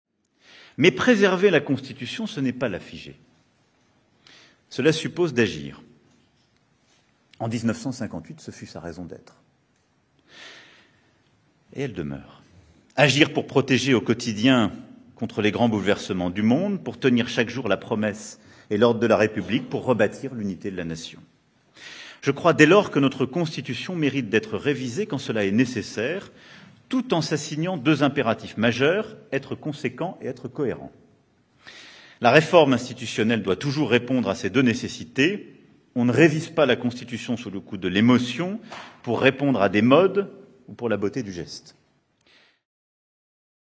Macron: on "ne révise pas la Constitution sous le coup de l'émotion" | AFP Extrait
"On ne révise pas la Constitution sous le coup de l’émotion, pour répondre à des modes ou pour la beauté du geste", déclare le président Emmanuel Macron dans son discours au Conseil Constitutionnel.